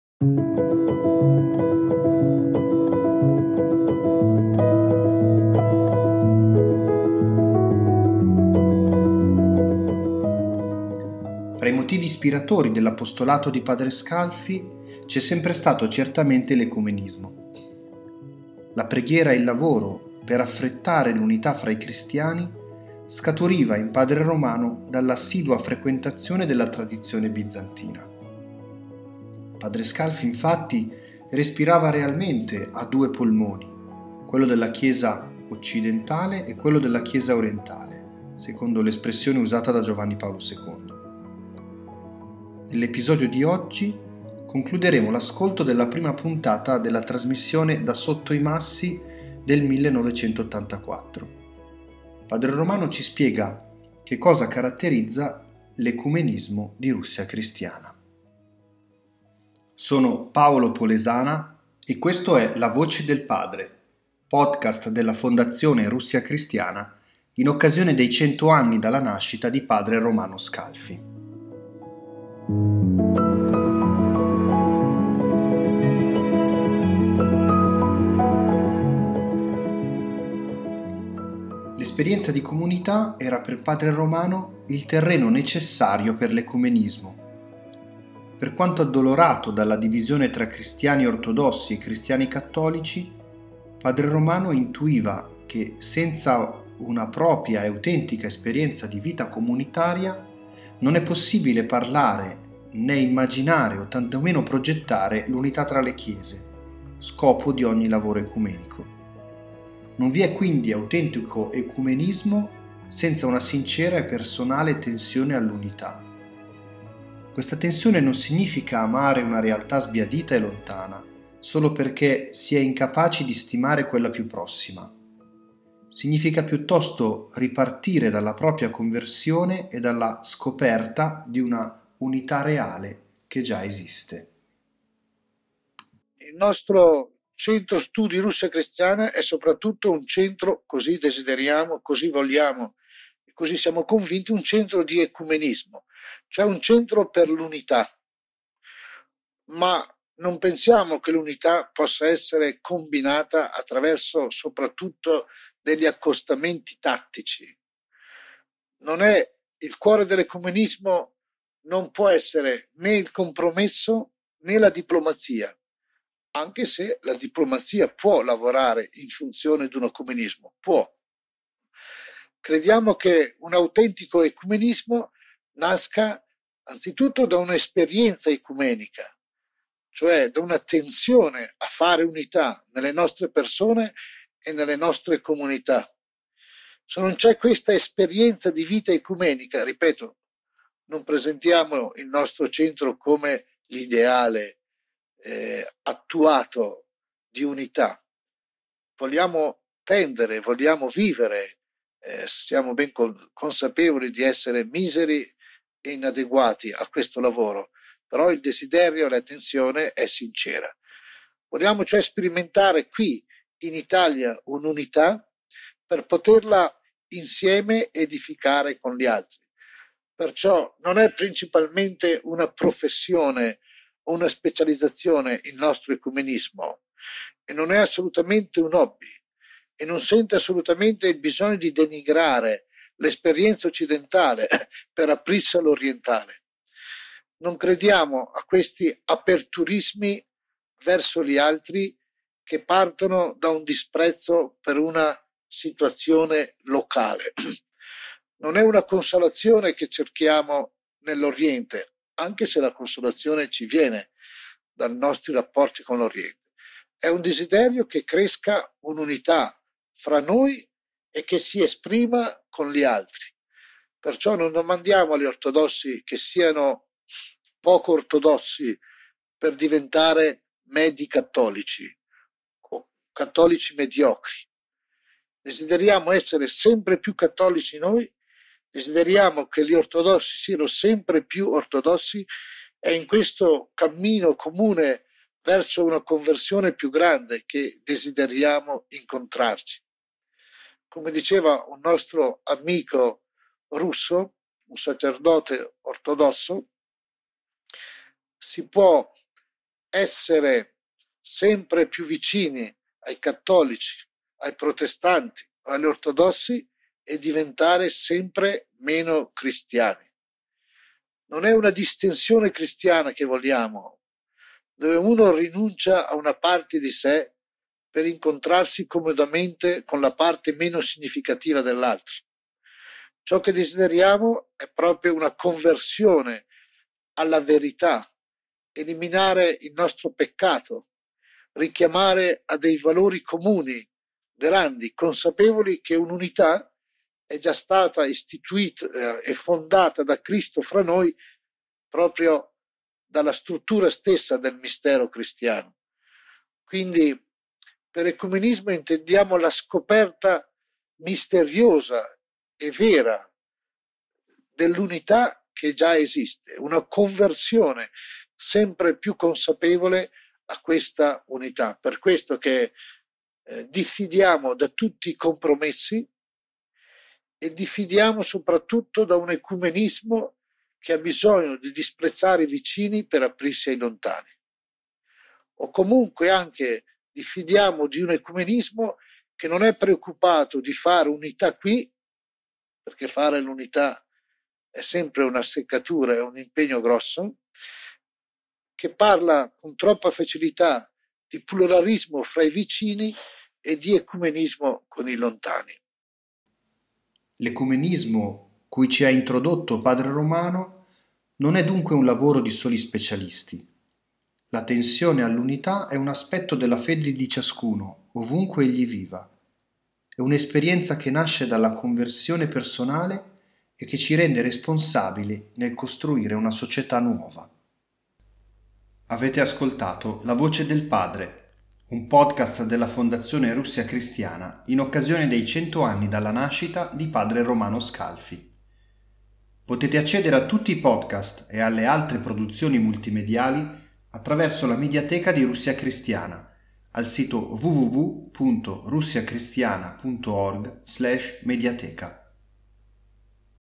Episodio 6: L'ecumenismo nell'esperienza
Fra i motivi ispiratori dell’apostolato di padre Scalfi c’è sempre stato certamente l’ecumenismo. Padre Romano, infatti, respirava realmente «a due polmoni», quello della Chiesa occidentale e quello della Chiesa orientale, secondo l’espressione usata da Giovanni Paolo II. Da questa piena vitalità nello spirito scaturiva il suo lavoro per affrettare l'unità fra i cristiani.

La voce è di Paolo Polesana.
I testi sono di Paolo Polesana e Amata Schira.